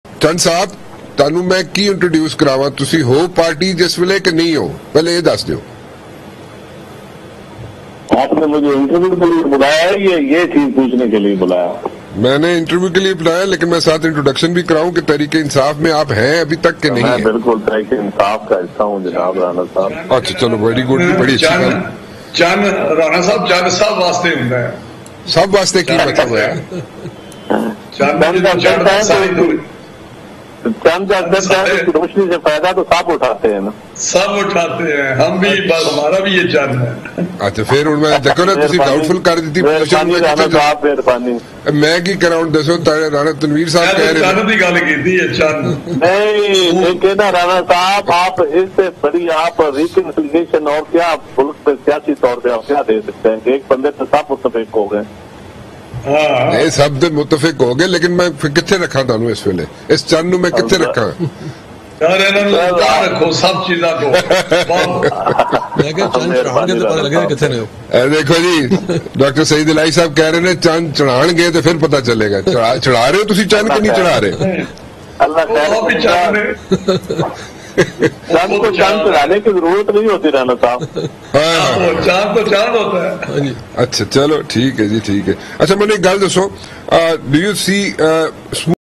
Nadeem Afzal Chan Kiya PTI Ka Hissa Hain Ya Nahi?| Aaj News
Host: Rana Mubashir

Guests:
- Kamran Murtaza (Member of the Senate of Pakistan)
- Rana Tanveer Hussain (Member of the National Assembly of Pakistan)
- Dr. Saeed Elahi
- Nadeem Afzal Chan (Former Member of the National Assembly of Pakistan)

Topics:
-PPP Aur PDM Ka Alag Alag Long March
-Murree Incident
-The Long March
-PPP And PDM
-Opposition United
-Conflict or Not?
-Year 2022 and Elections
-Possible?
-The Next 3-Months
-Important?
-Economic Crisis
-The Changing Year
-Opposition And Agenda
-On One Point?
-Political Pressure
-The Consequence?
-Table Agenda
-Imran Khan

Aaj TV Official:
shorturl.at/iBDNX